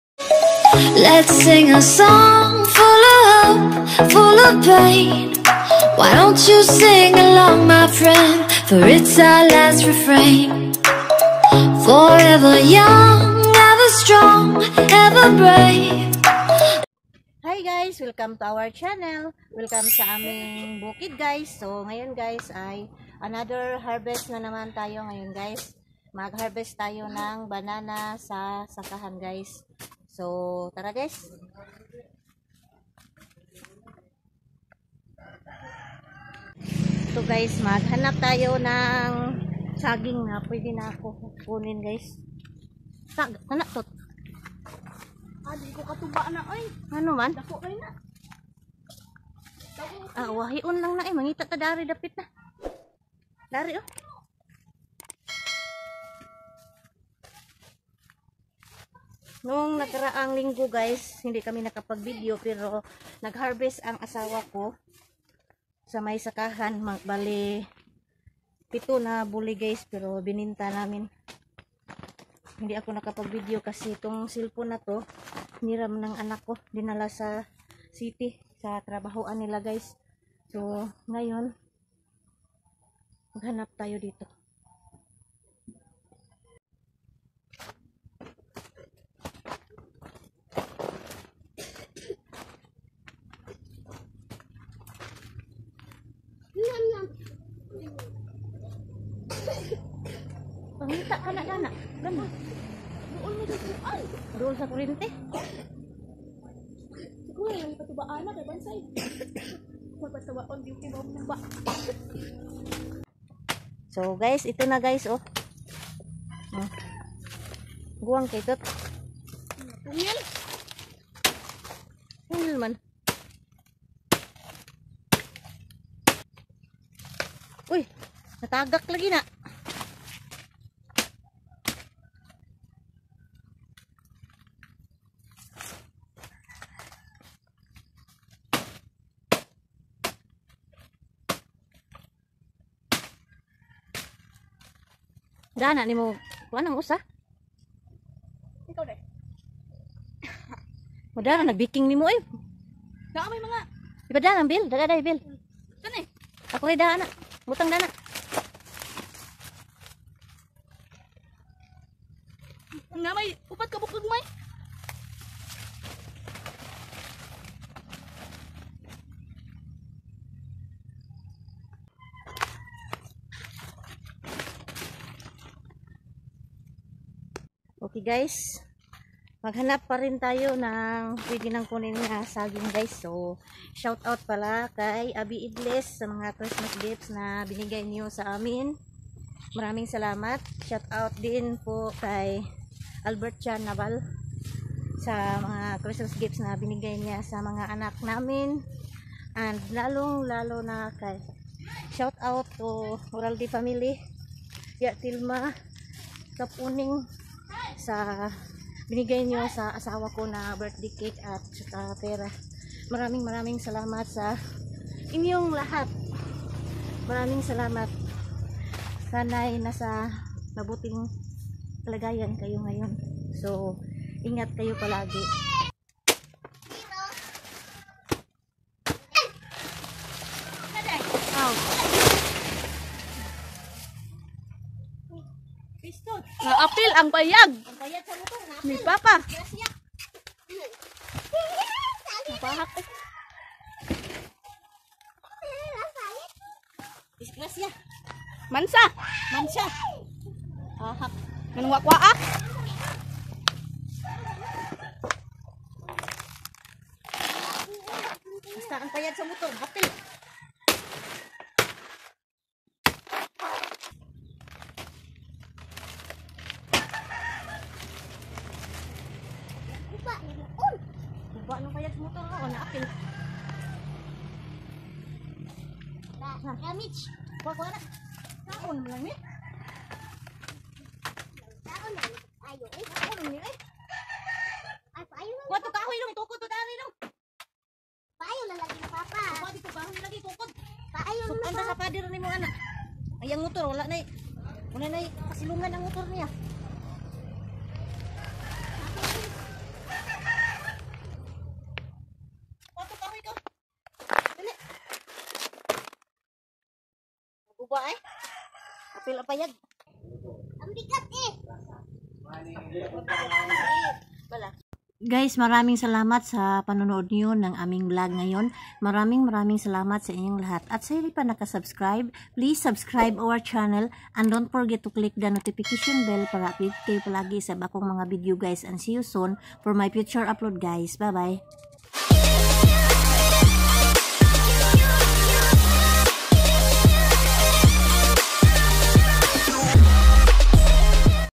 HARVESTING BANANA (KARDAVA) AT THE FARM.
Welcome to our mountain guys. In this vlog we're here at the farm to harvest banana.